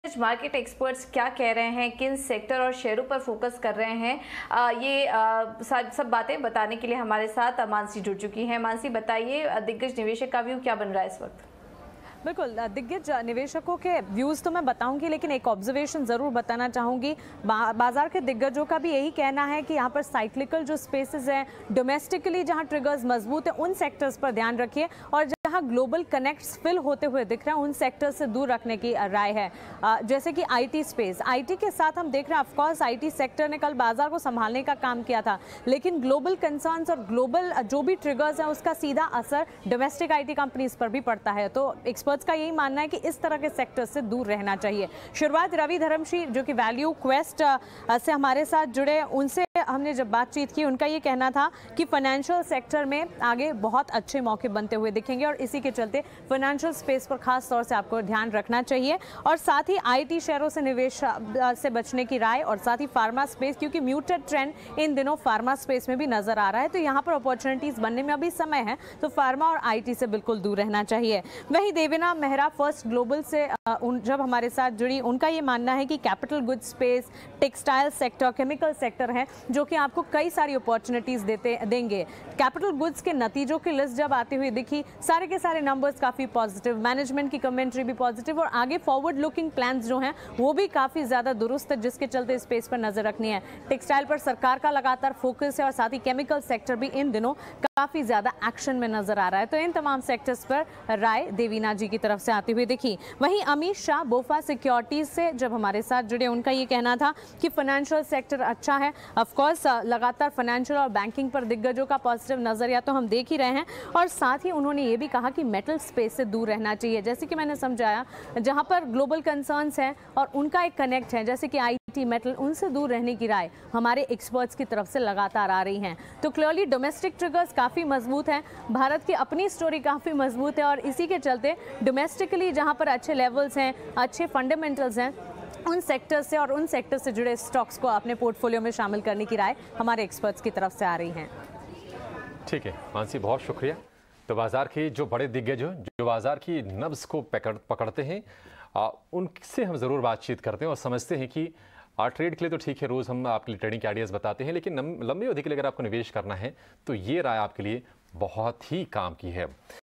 Share Market में किन Sector और Share करें फोकस, जानिए Market पर Experts की राय
ValueQuest Investment Advisors की रवि धरमशी का कहना है कि फाइनेंस सेक्टर पर बहुत अच्छे मौके बनते हुए दिखाई देंगे। फार्मा और IT शेयरों में निवेश से बचने की राय है।
First Global की देवीना मेहरा का कहना था कि कैपिटल गुड्स टेक्सटाइल केमिकल सेक्टर पर बुलिश करना सही है।
BofA Securities के अमीश शाह का कहना है कि फाइनेंशियल सेक्टर काफी अच्छा है। लेकिन उन्होंने यह कहा कि मेटल शेयरों में निवेश से बचे रहना चाहिए।